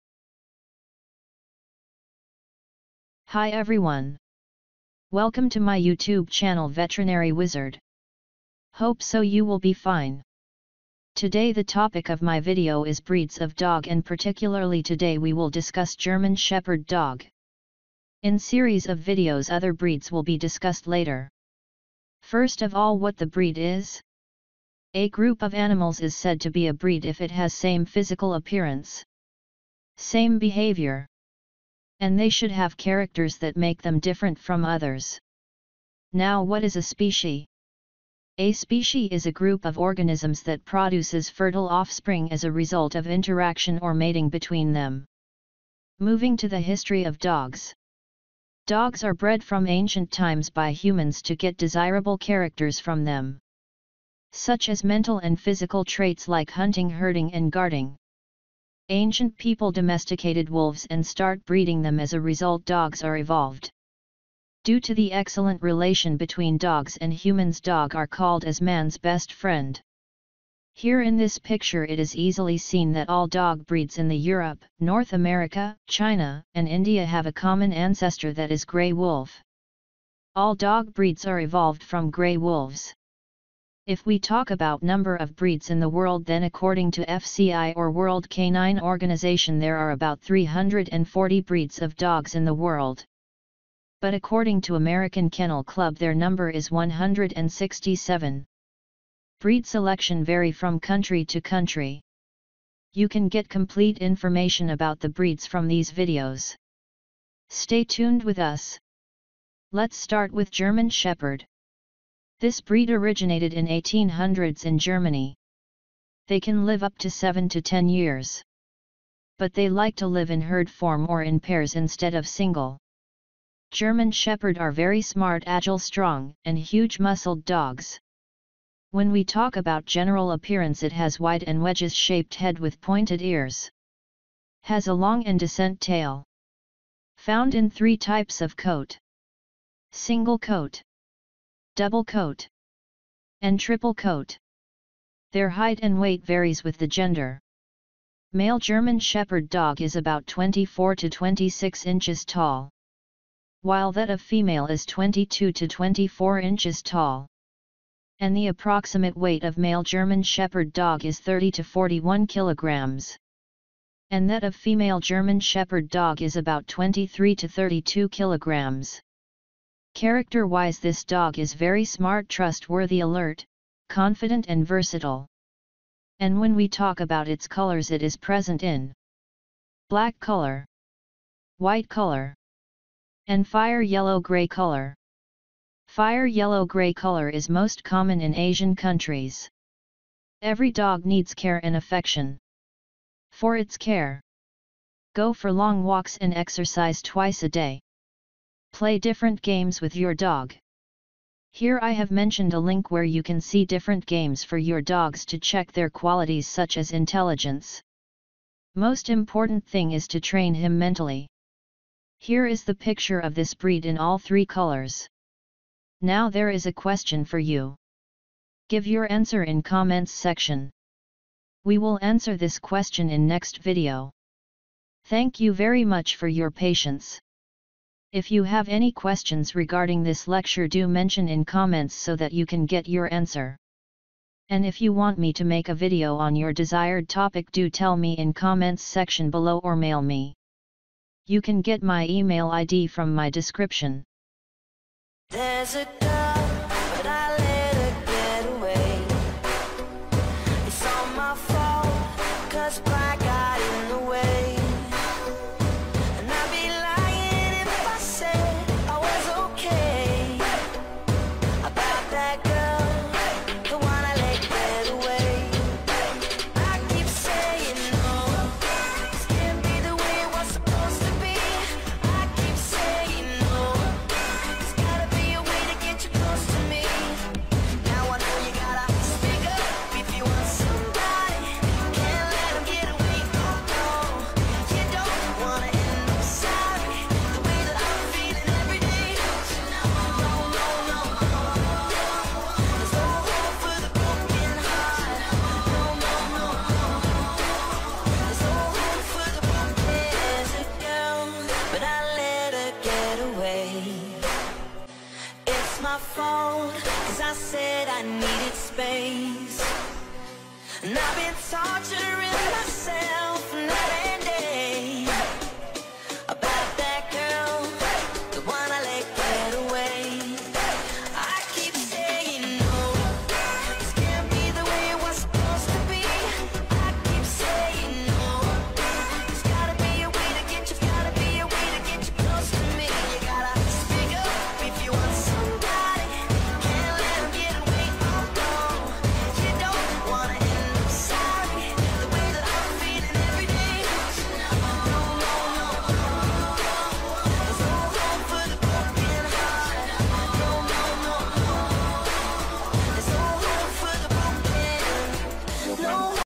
Breeds of Dogs ( German Shepherd Dog )
This video is about the German Shepherd Dogs
You can get Complete information about this breed from this video